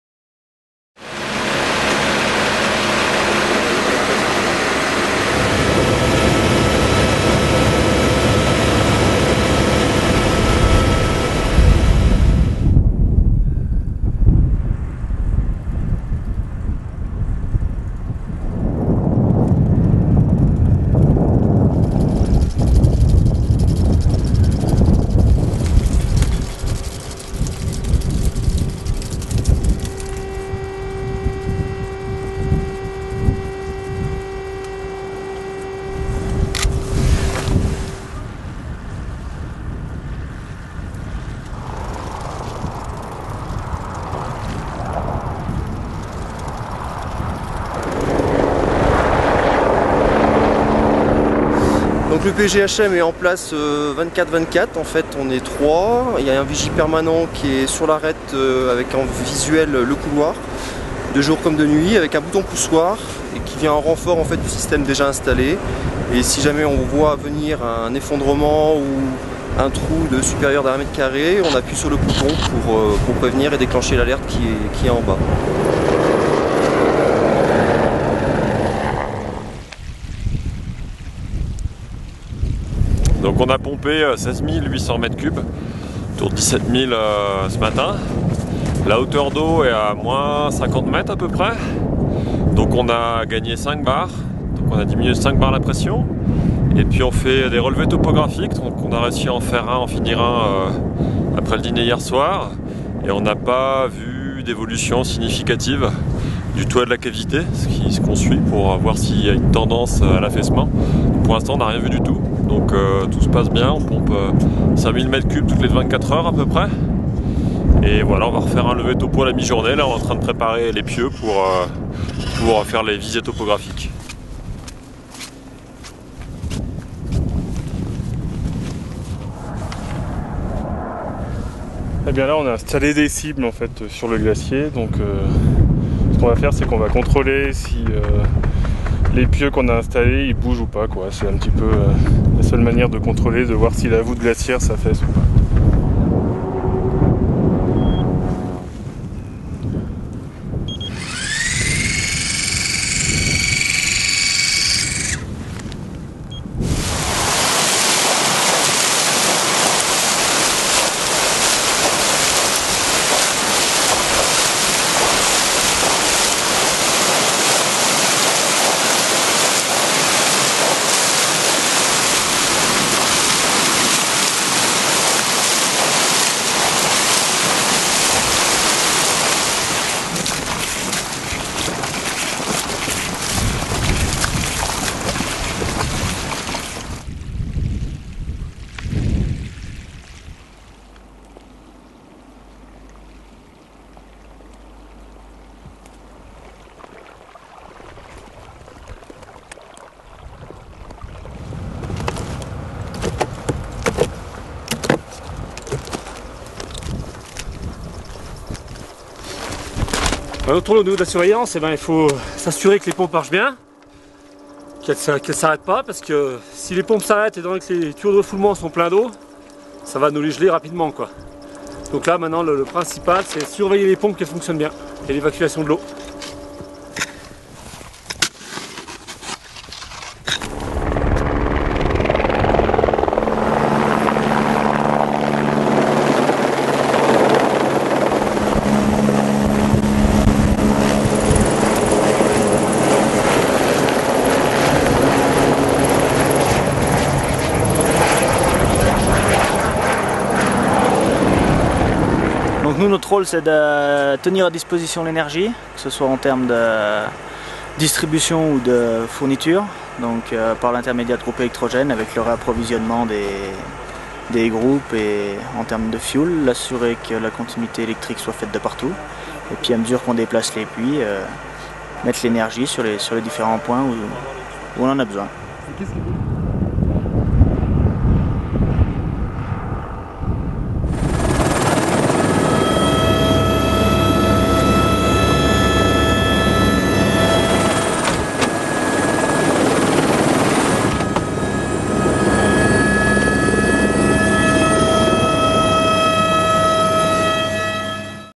Poche d'eau du Glacier de Tête Rousse Saint-Gervais Mont-Blanc environnement sécurité montagne
Septembre 2010, environnement, travaux montagne... Poche d'eau du Glacier de Tête Rousse, Saint-Gervais Mont-Blanc... Quelques images du pompage de la poche d'eau du glacier de Tête Rousse... Cela se vide doucement... Avec Matthieu Robin de PGHM de Chamonix, Nicolas Karr ( Office National des Forêts RTM ), Christel Bottelier Curtet ( Boma Construction ), Cédric Gauthier ( EGE Gramari )...